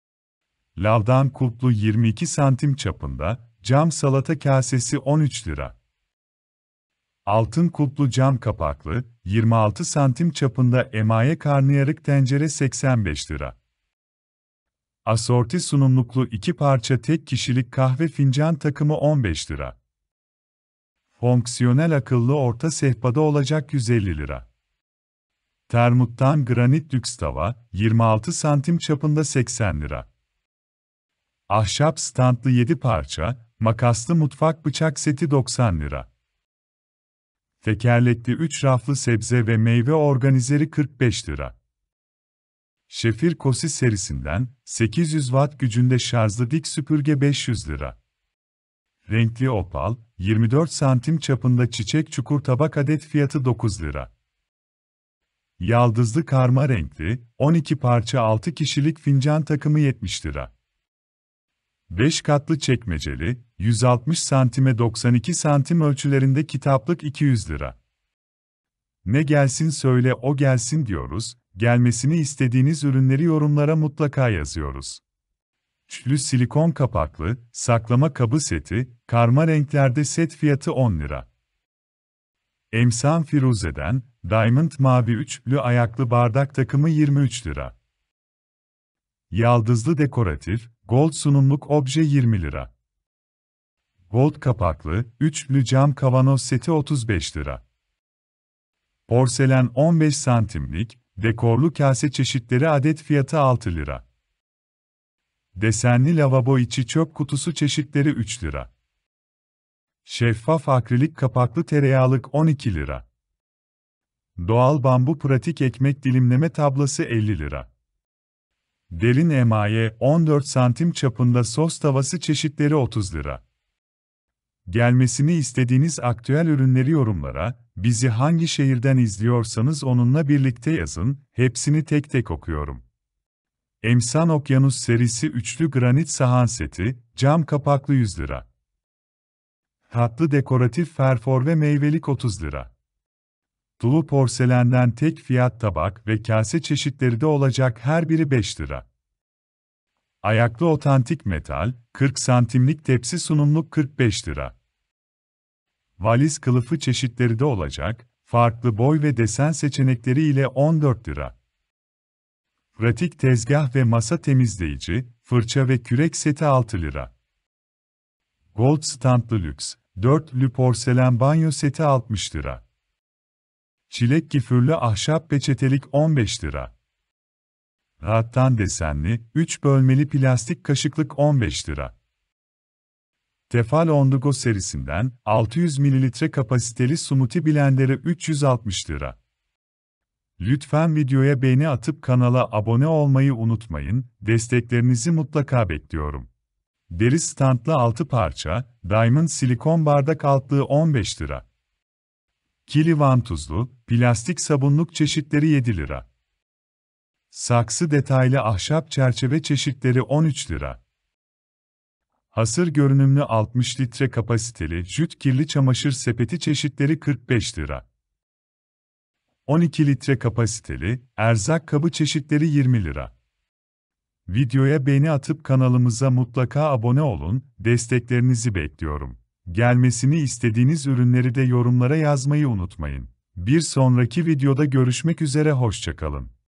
A101 AKTÜEL YARIN | ARALIK 2022 | A101 PAZAR ARABASI İNDİRİMLERİ
A101 Market Yarın Aktüel kataloğu. A101 ARALIK 2022 kataloğu çıktı. fırsatlarla dolu A101 PAZAR ARABASI İNDİRİMLERİ kampanyalı aktüel ürünleri göreceğiz.